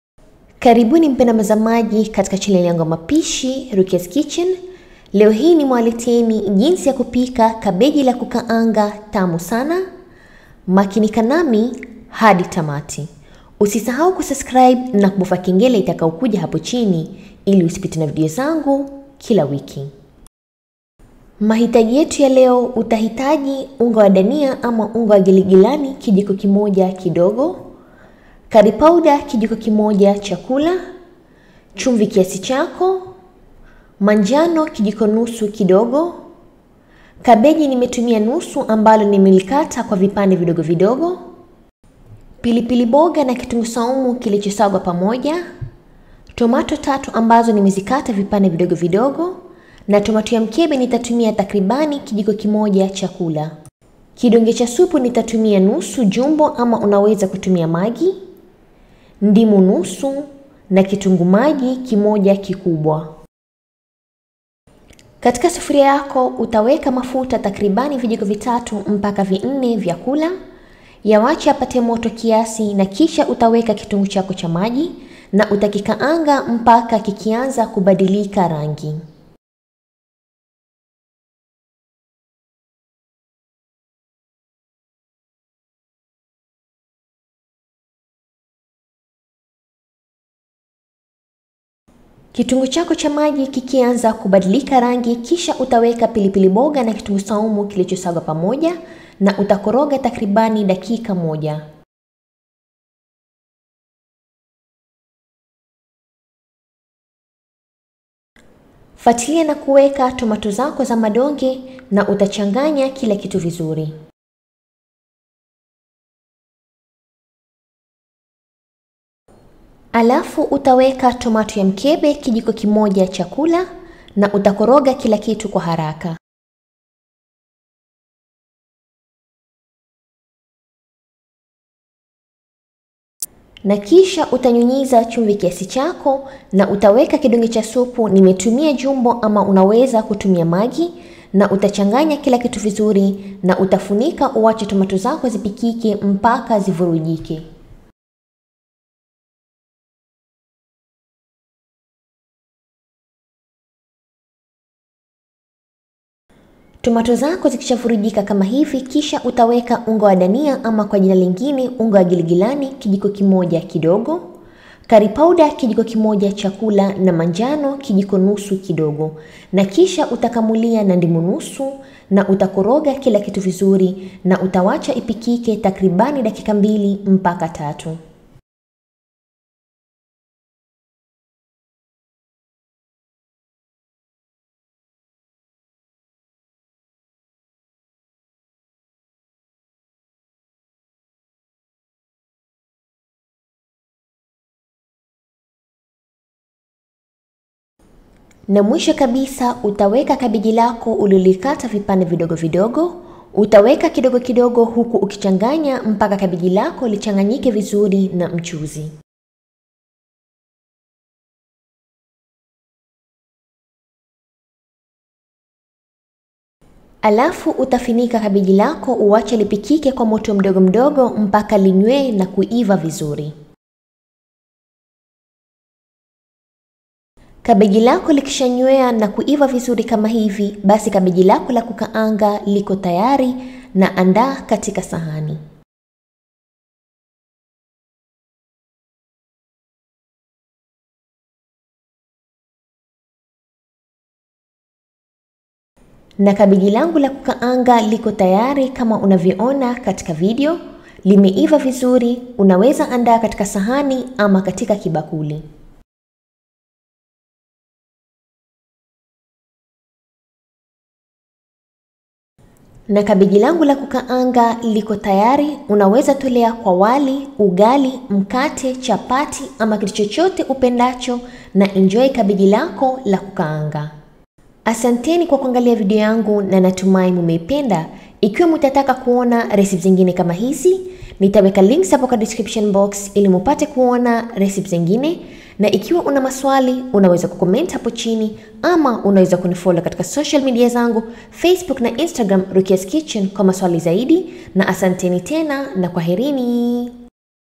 Kabeji lakukaanga | jinsi yakupika kabeji lakukaanga tamu sana | Mapishi rahisi ya Kabichi .
Cookpad: Rukias kitchen

Mahitaji :-
Kabichi
Tomato 3
Tomato  mkebe  kijiko  1 cha kula
Dania  ama  Giligilani  ya  unga  kijiko  kidogo
Curry powder  kijiko  kidogo
Manjano  kijiko nusu  kidogo
Chumvi
Jumbo
Kitunguu  maji 1
Kitunguusaumu na  pilipilboga  iliyosagwa
Ndimu  nusu